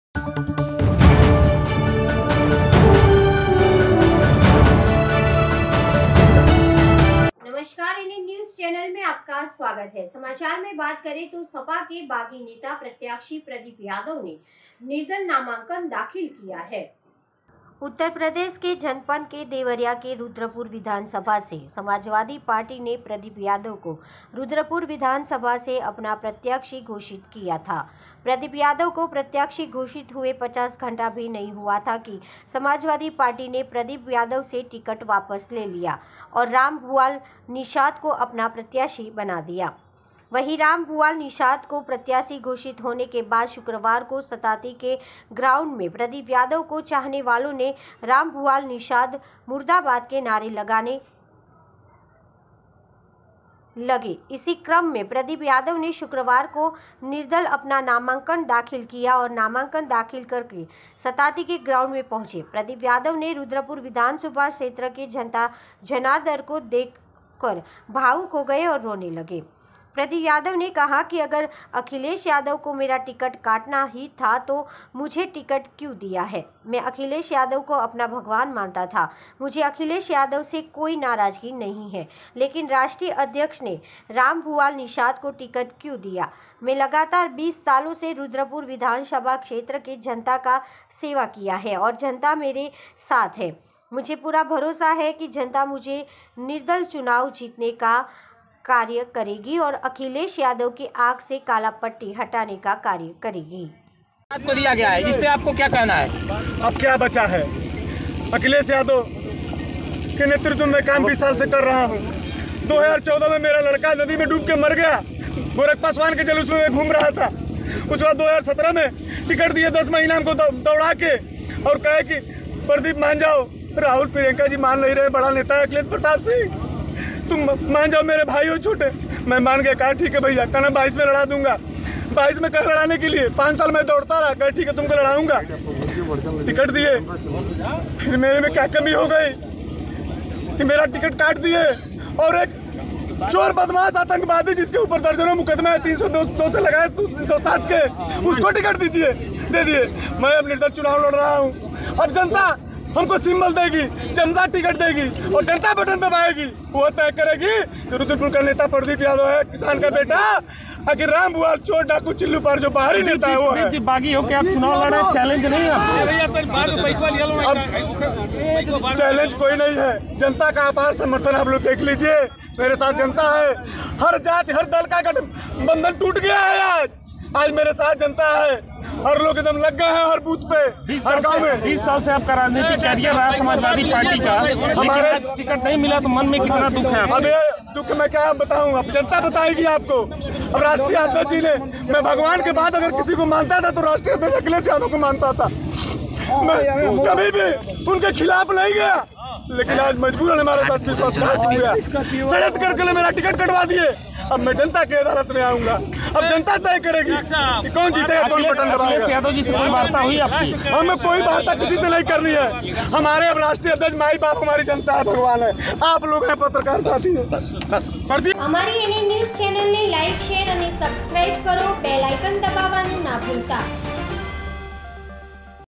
सपा के बागी नेता प्रत्याशी प्रदीप यादव ने निर्दल नामांकन दाखिल किया
उत्तर प्रदेश के जनपद देवरिया के रूद्रपुर विधानसभा से समाजवादी पार्टी ने प्रदीप यादव को रूद्रपुर विधानसभा से अपना प्रत्याशी घोषित किया था।प्रदीप यादव को प्रत्याशी घोषित हुए पचास घंटा भी नही हुआ था कि समाजवादी पार्टी ने प्रदीप यादव से टिकट वापस ले लिया और रामभुआल निषाद को अपना प्रत्याशी बनाया दिया।

वही रामभुआल निषाद को प्रत्याशी घोषित होने के बाद शुक्रवार को सताती के ग्राउंड में प्रदीप यादव को चाहने वालो ने रामभुआल निषाद मुर्दाबाद के नारे लगाने लगे।इसी क्रम में प्रदीप यादव ने शुक्रवार को निर्दल अपना नामांकन दाखिल किया है।नामांकन दाखिल कर सताती के ग्राउंड में पहुचे प्रदीप यादव ने रूद्रपुर विधानसभा क्षेत्र कि जनता जनार्दन को देखकर भावुक हो गए और रोने लगे।

प्रदीप यादव ने कहा कि अगर अखिलेश यादव को मेरा टिकट काटना ही था तो मुझे टिकट क्यो दिया है।मैं अखिलेश यादव को अपना भगवान मानता था।मुझे अखिलेश यादव से कोई नाराजगी नही है लेकिन राष्ट्रीय अध्यक्ष ने रामभुआल निषाद को टिकट क्यो दिया।मैं लगातार 20 सालो से रूद्रपुर विधानसभा क्षेत्र की जनता का सेवा किया है और जनता मेरे साथ है।मुझे पूरा भरोसा है कि जनता मुझे निर्दल चुनाव जीतने का कार्य करेगी और अखिलेश यादव के आँख से काला पट्टी हटाने का कार्य करेगी।

प्रेस रिपोर्टर राम चौधरी
लोकेशन उत्तर प्रदेश के देवरिया से